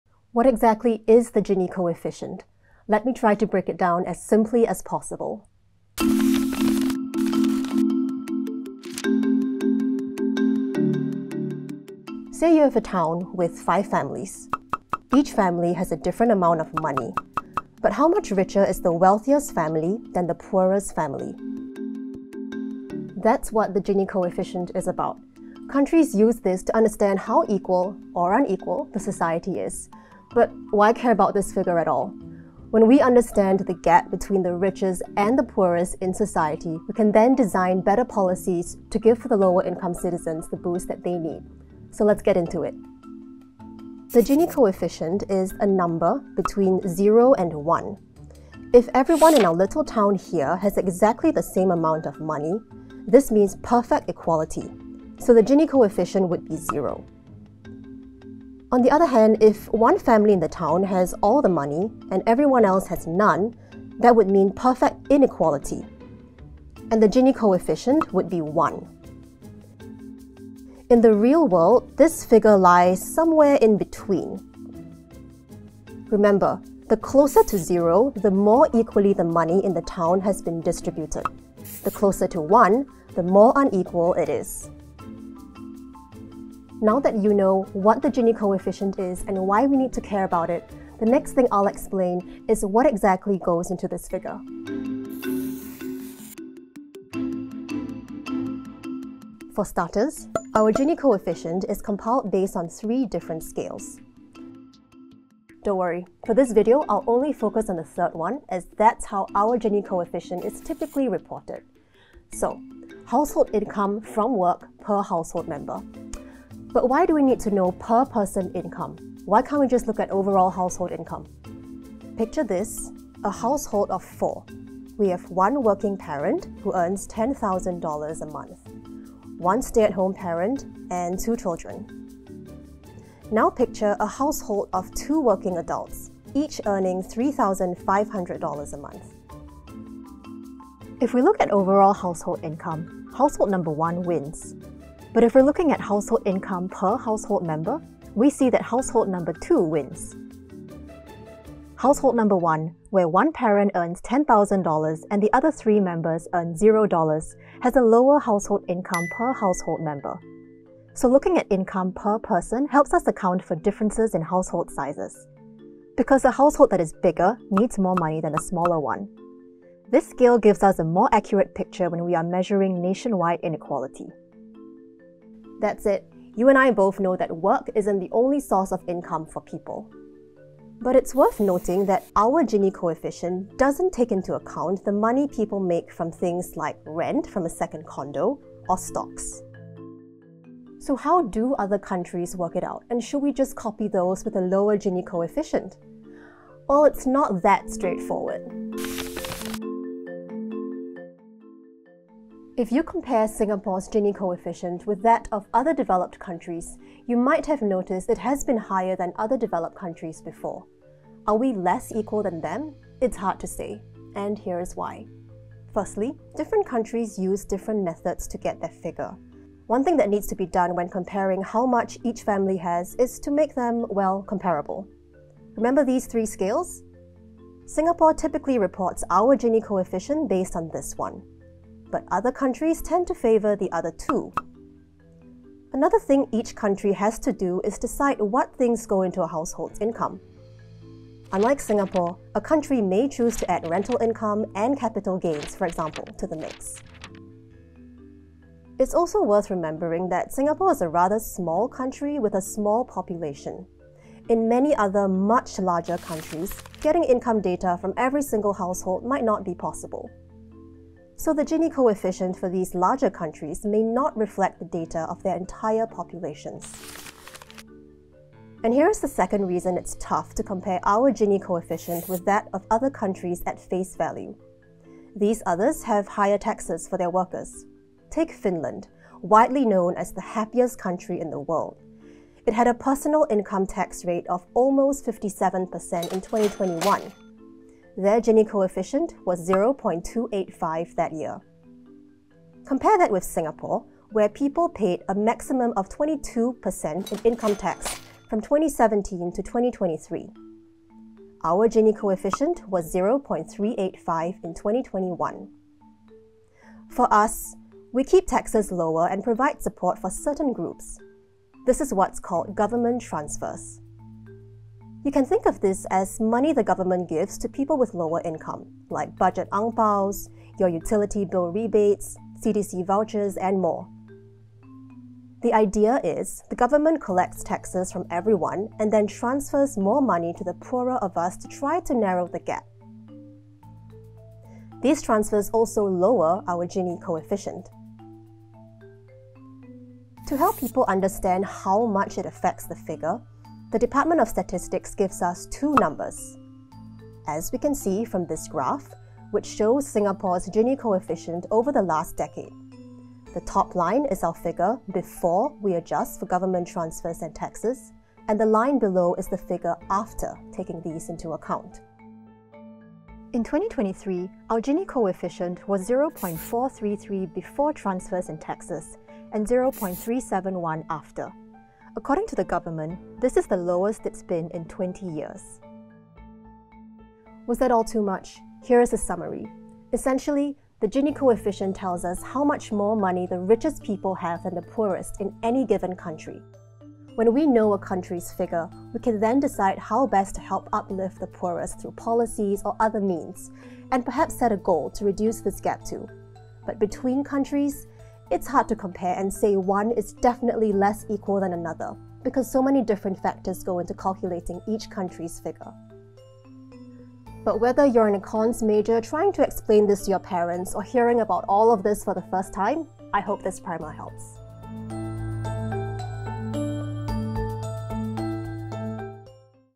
What is the Gini coefficient, and what is it used for? | CNA Explains
What is the Gini coefficient, and what is it used for? And how does Singapore work out its Gini coefficient? If you've ever struggled to understand this concept, CNA's Grace Yeoh breaks it down for you - with handy illustrations.